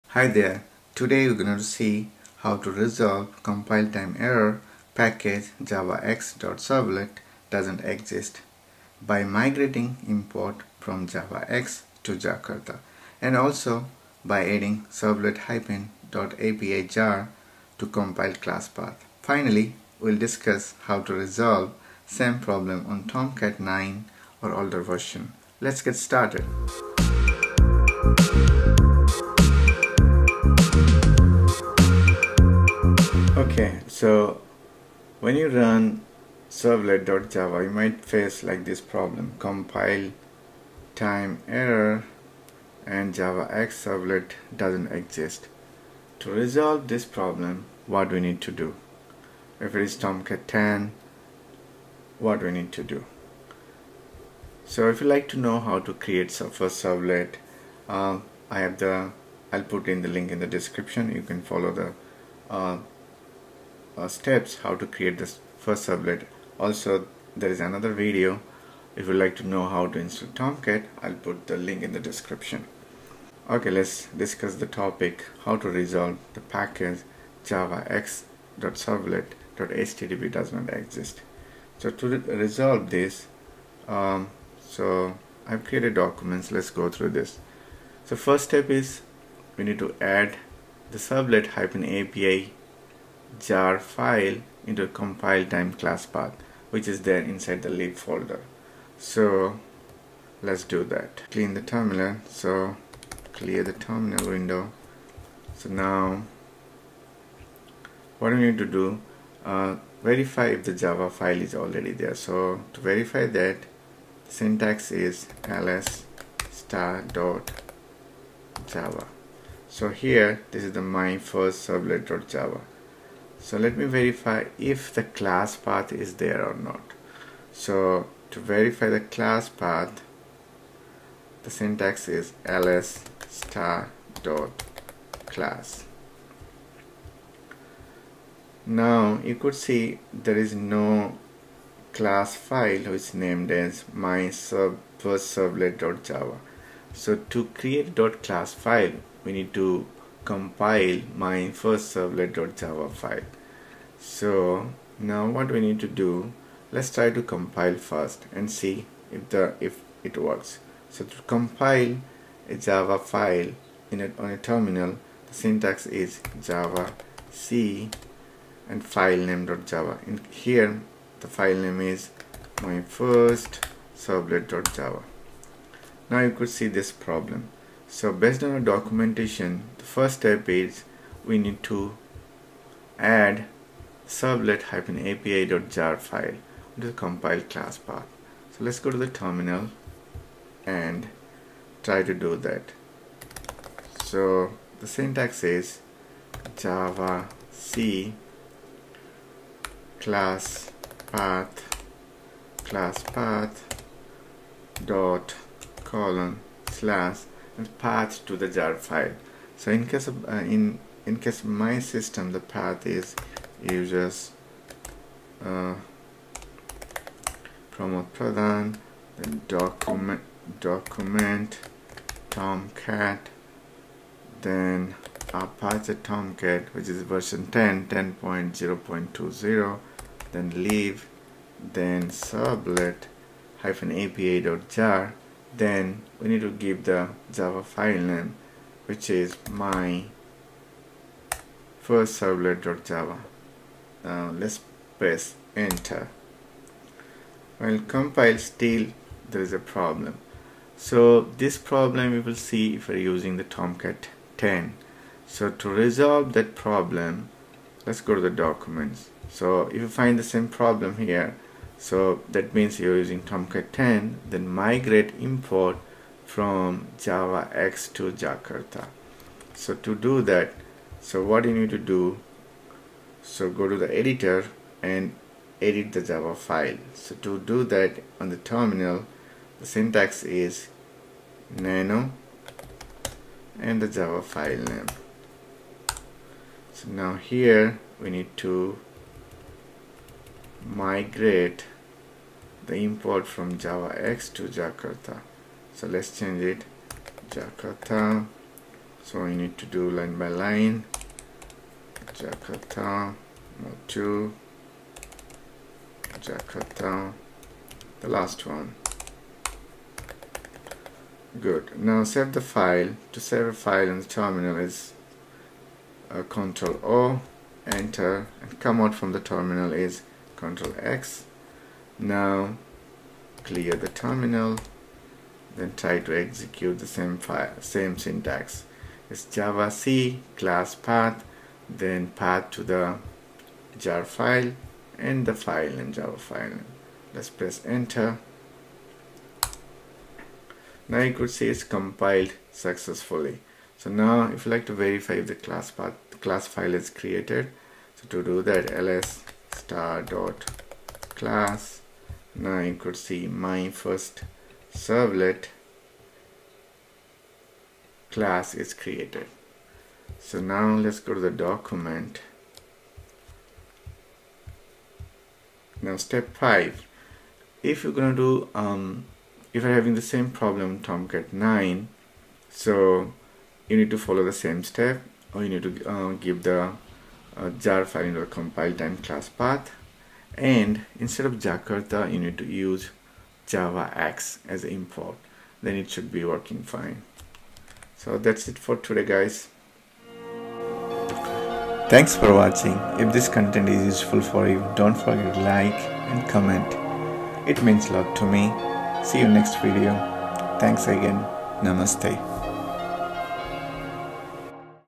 How to fix package javax.servlet does not exist in Tomcat 10.
You ll learn how to add the path to Tomcat's /lib/servlet-api. jar file to the compile time classpath and migrate import from javax to jakarta.

This video will help  to fix javax servlet does not exist? ,
What is javax servlet http package?How do I fix javax servlet does not exist?
Where is javax servlet package?
𝌞 TABLE OF CONTENT
0:00 Introduction
01:30 Add servlet-api. jar to compile classPath
04:30 Migrate import from Javax to Jakarta
06:10 Javax compile error solution for Tomcat9 or older version
06:30 Thank You!!